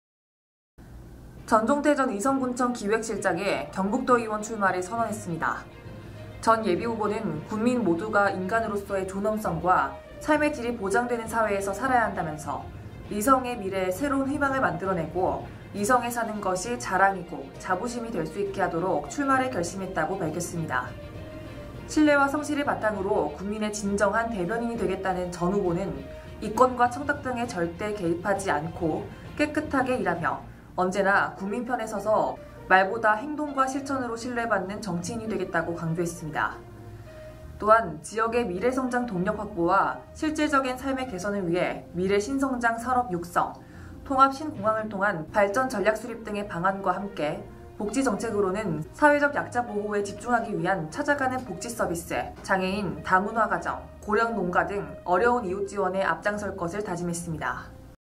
전종태 전 의성군청 기획실장, 경북도의원 출마 선언
전종태 전 의성군청 기획실장(국민의힘)이 경북도의원(의성군 제1선거구) 출마를 선언했다.

전 예비후보는 “군민 모두가 인간으로서의 존엄성과 삶의 질이 보장되는 사회에서 살아야한다”면서 “의성의 미래에 새로운 희망을 만들어내고, 의성에 사는 것이 자랑이고 자부심이 될 수 있게 하도록 출마를 결심했다”고 밝혔다.

“신뢰와 성실을 바탕으로 군민의 진정한 대변인이 되겠다”는 전 후보는 “이권과 청탁 등에 절대 개입하지 않고 깨끗하게 일하며 언제나 군민 편에 서서 말보다 행동과 실천으로 신뢰받는 정치인이 되겠다”고 강조했다.

또 지역의 미래성장동력 확보와 실질적인 삶의 개선을 위해 ‘미래 신성장 산업육성’, ‘통합신공항을 통한 발전 전략 수립’ 등의 방안과 함께 복지정책으로는 ‘사회적 약자 보호에 집중하기 위한 찾아가는 복지 서비스’, ‘장애인, 다문화가정, 고령농가 등 어려운 이웃 지원’에 앞장설 것을 다짐했다.